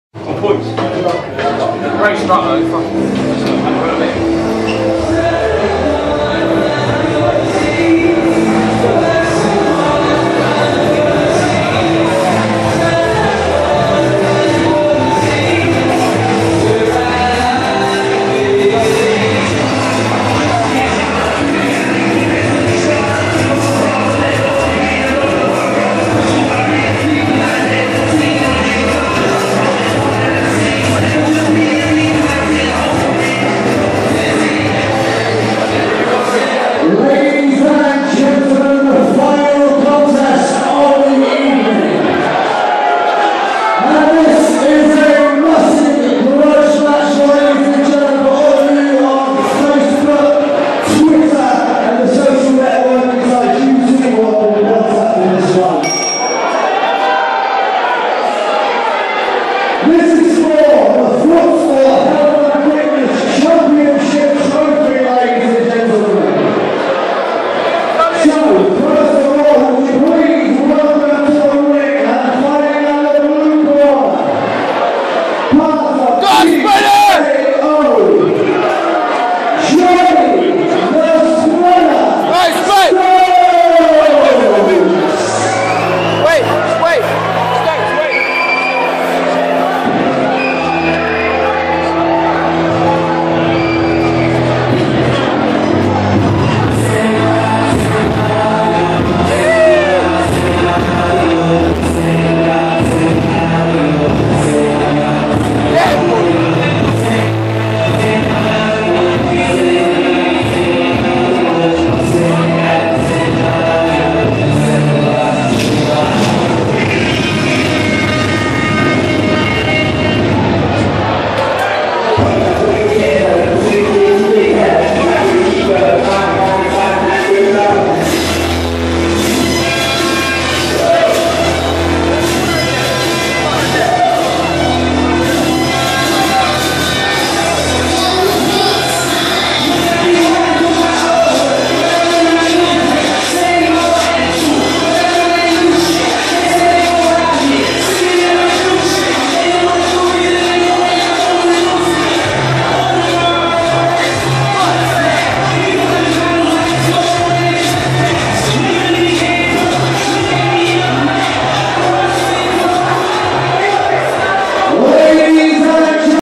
Jonnie ''The Spreader'' Stokes ring entrance
The Spreader making his way to the ring for his clash with Lee Hedges. Another dazzling display and the boy is now 3-0!